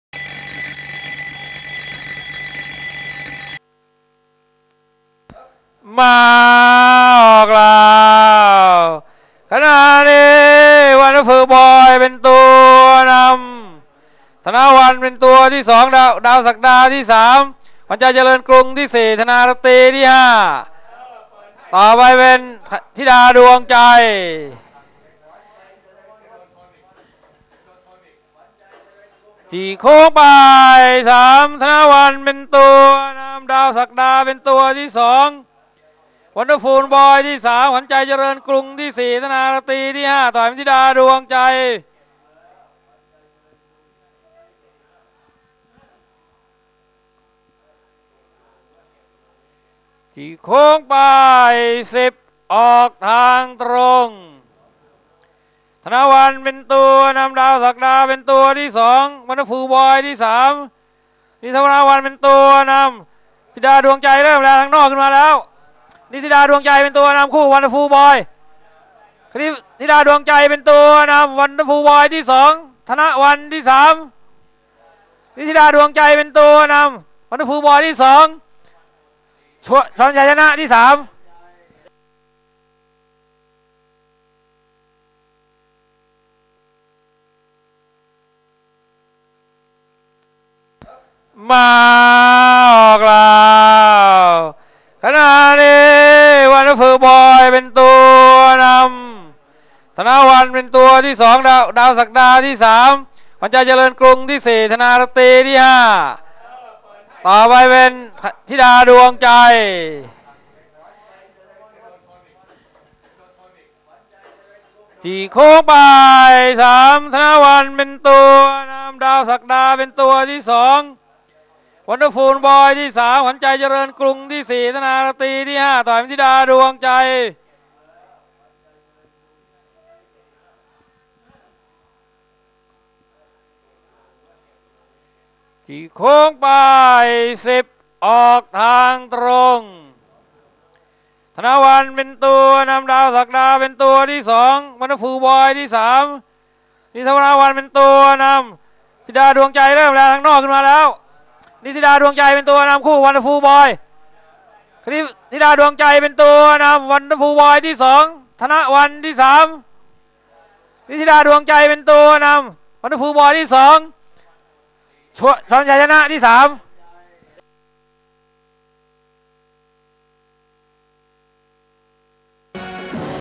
สนามม้านางเลิ้ง 29กค.55 เที่ยว 6 ชั้น 6
วันมงคลขี่ธิดาดวงใจเบอร์ 4 ชนะ เอกอนันต์ขี่วันเดอร์ฟูลบอยเบอร์ 3 และ สมชาติขี่ชมชัยชนะเบอร์ 8